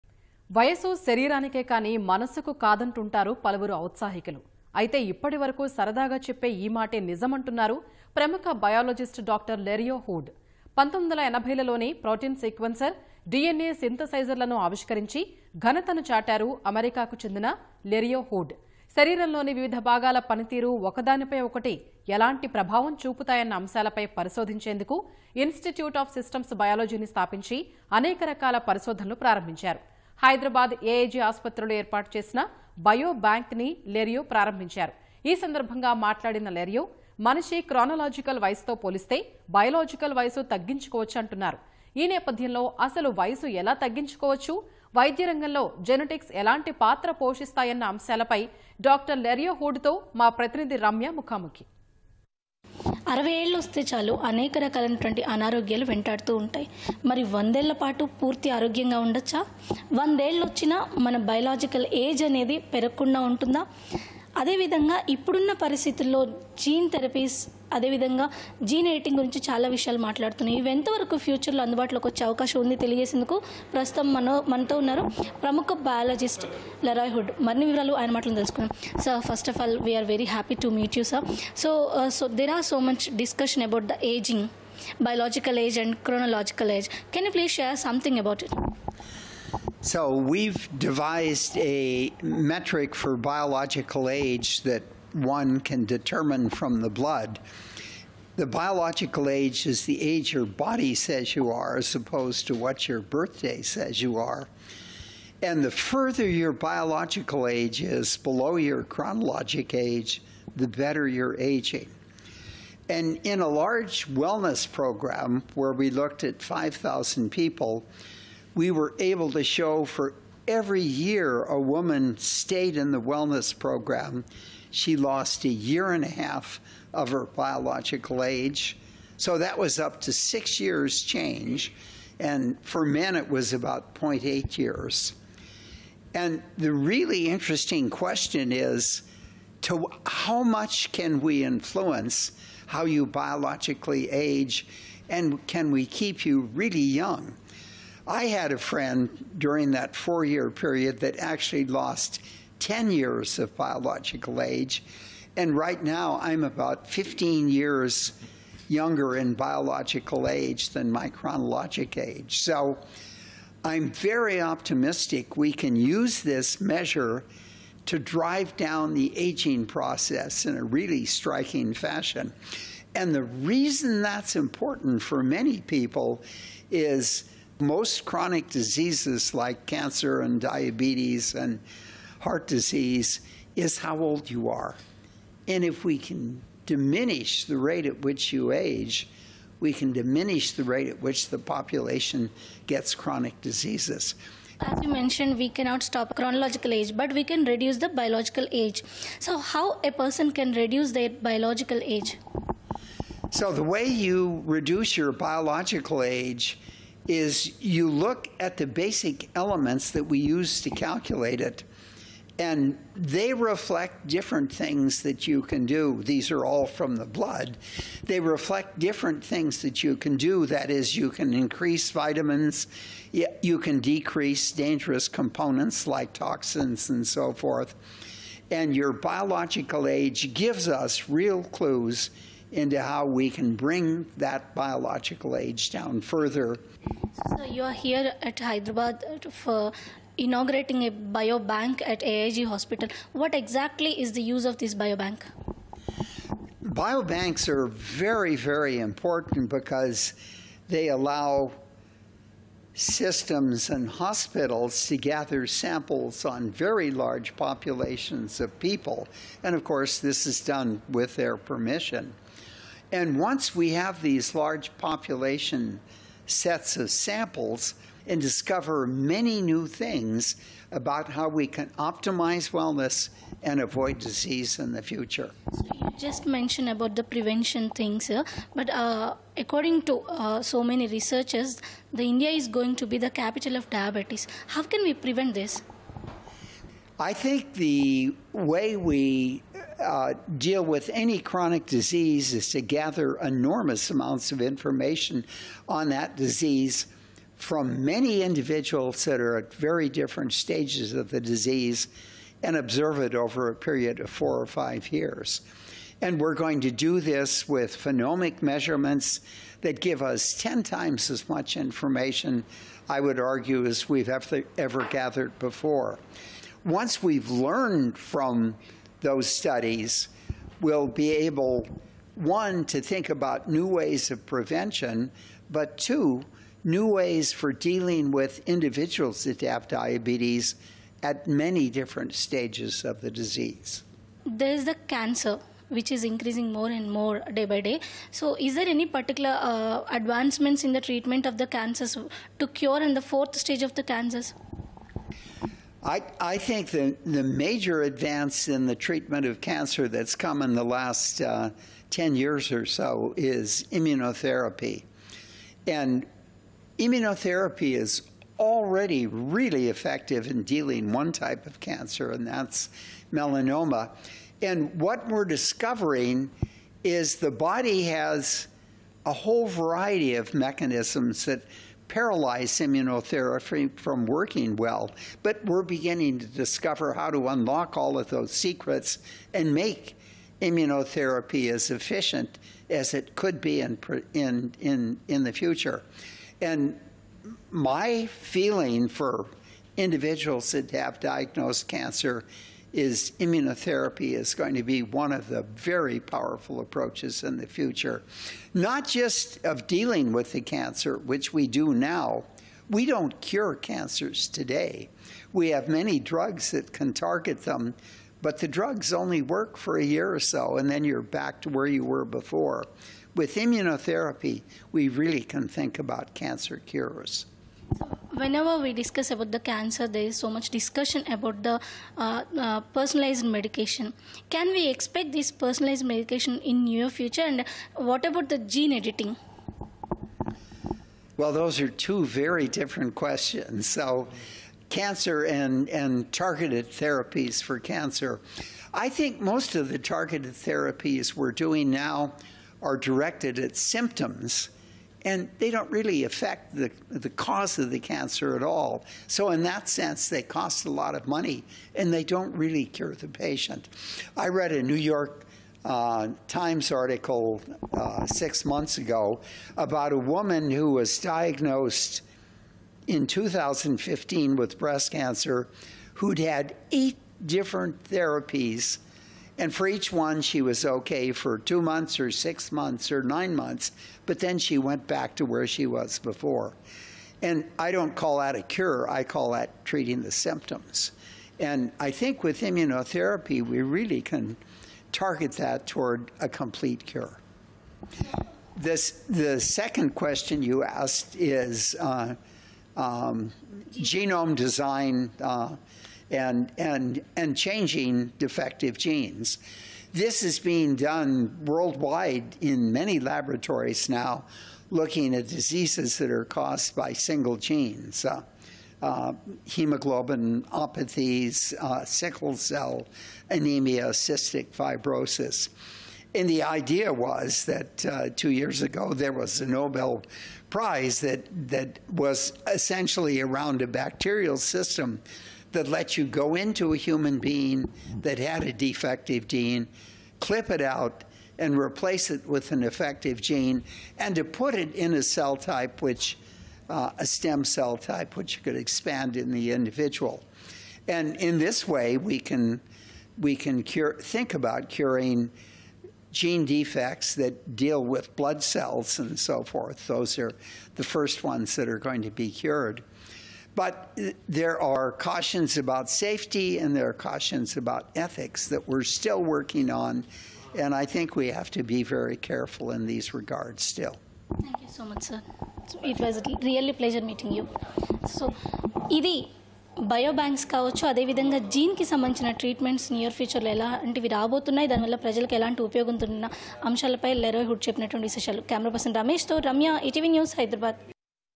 Bio Bank to Lessen Biological Age Compared to Chronological | Dr Leriohood Interview || Idi Sangathi
వయసు శరీరానికే కాని మనసు కాదంటుంటారు పలువురు ఔత్సాహితులు . ఇప్పటి వరకు సరదాగా చెప్పే ఈ మాటే నిజమంటున్నారు ప్రముఖ బయాజిస్ట్ డాక్టర్ లెరియో హూడ్. 1980లలోనే ప్రోటీన్ సీక్వెన్సర్, డీఎన్ఏ సింథసైజర్ లను ఆవిష్కరిం ఘనతను చాటారు అమెరికాకు చెందిన లెరియో హూడ్. శరీరంలోని వివిధ భాగాల పనితీరు ఒకదానిపై ఒకటి ఎలాంటి ప్రభావం చూపుతాయన్న అంశాలపై పరిశోధించేందుకు గాను ఇనిస్టిట్యూట్ ఆఫ్ సిస్టమ్స్ బయాలజి ని స్థాపించి అనేక రకాల పరిశోధనలకు ప్రారంభించారు. హైదరాబాద్ ఏఐజీ ఆస్పత్రిలో ఏర్పాటు చేసిన బయో బ్యాంక్ లెరియో ప్రారంభించారు. ఈ సందర్భంగా మాట్లాడిన లెరియో. మనిషి క్రానలాజికల్ వయసుతో పోలిస్తే. బయలాజికల్ వయసు తగ్గించుకోవచ్చంటున్నారు. ఈ నేపథ్యంలో అసలు వయసు ఎలా తగ్గించుకోవచ్చు. వైద్యరంగంలో జెనెటిక్స్ ఎలాంటి పాత్ర పోషిస్తుందన్న అంశాలపై డాక్టర్ లెరియో హూడ్ తో మా ప్రతినిధి రమ్య ముఖాముఖి